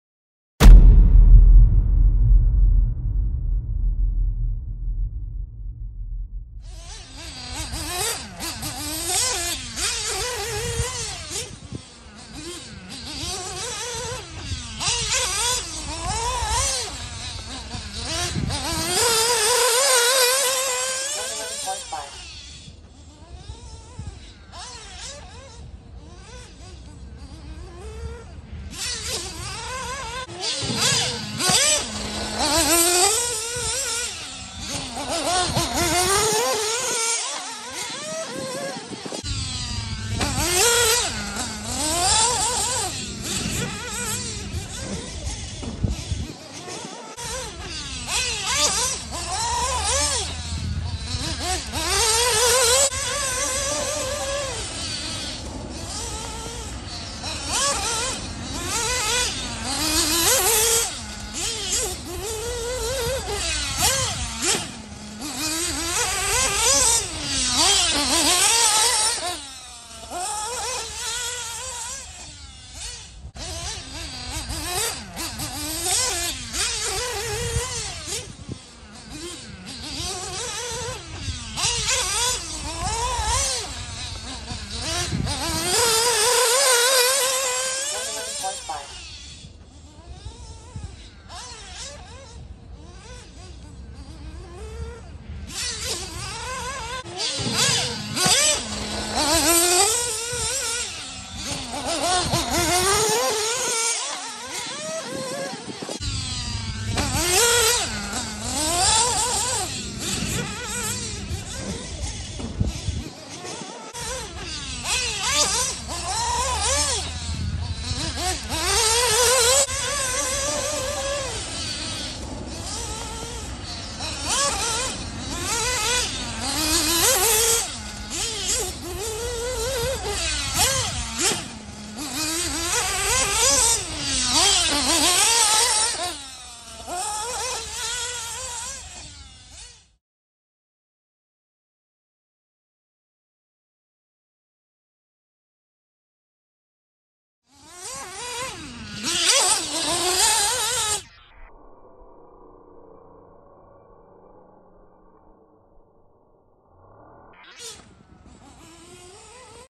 Tyler Jones Agama N1 Testing Texas USA
Join Tyler for a few laps as he tests out the new Agama N1 nitro buggy.